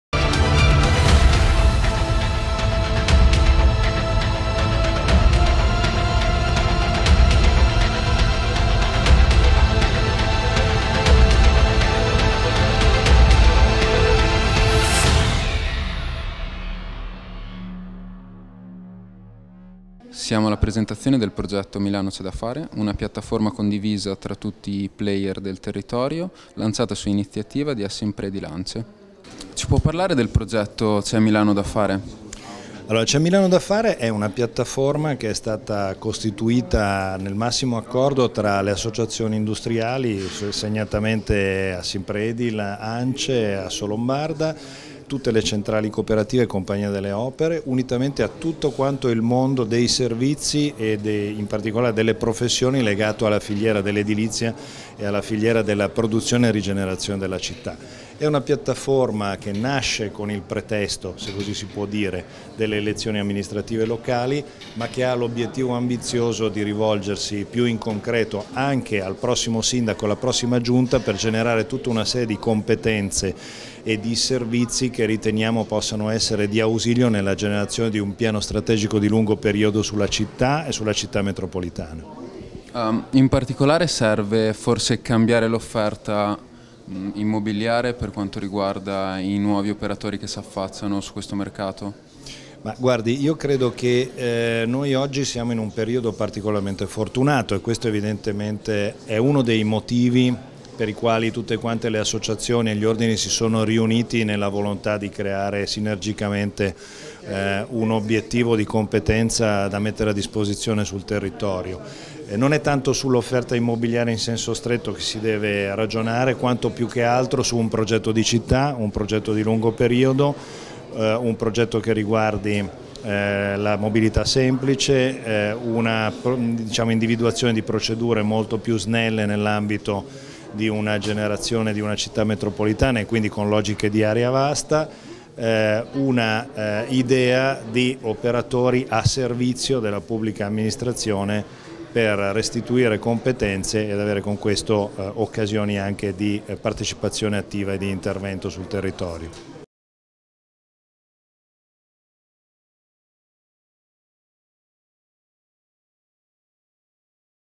Manifesto C'è Milano da fare, videointervista a Marco Dettori Assimpredil Ance
C'è da fare Milano è un progetto finalizzato alla costituzione di una piattaforma condivisa tra pubblico e privato per la progettazione degli interventi di edilizia sul territorio della nascente Città Metropolitana. Così Marco Dettori, presidente Assimpredil Ance, ha spiegato i punti chiave del Manifesto presentato all'Urban Center della Galleria Vittorio Emanuele.